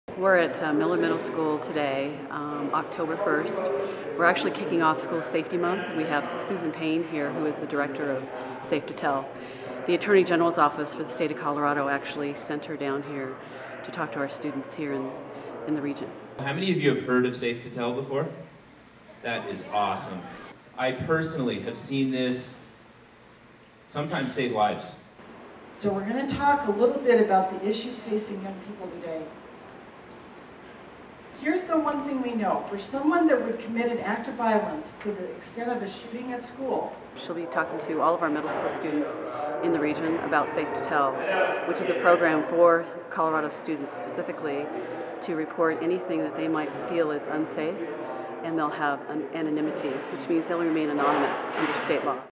safe2tell
Safe2Tell talk kicks off school safety month.